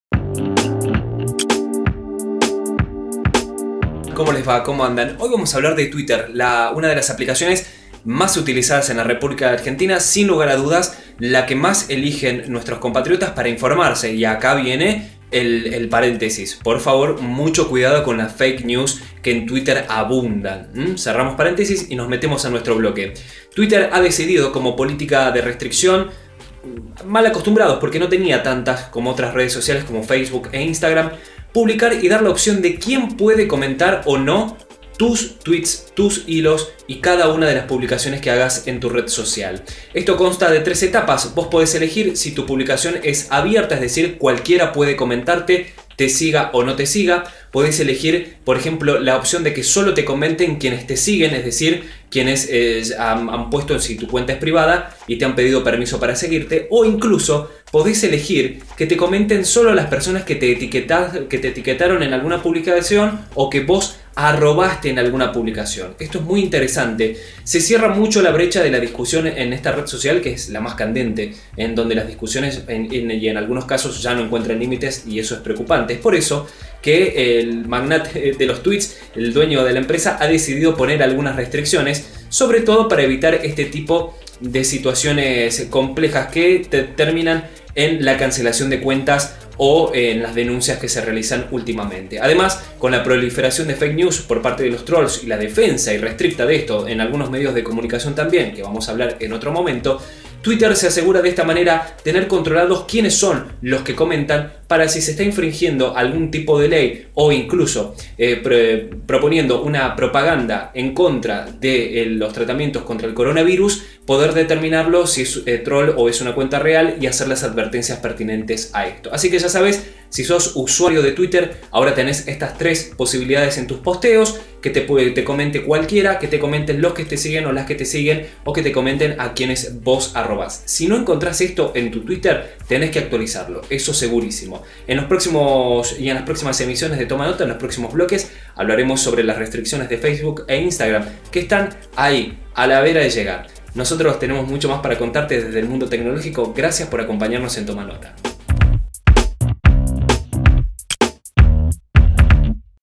La red del pajarito habilita restricciones en la interacción
Fernando Álvarez Méndez nos explica cómo funcionarán las nuevas restricciones que habilitó la red social Twitter. Quienes comentan los tweet o hilos de las cuentas.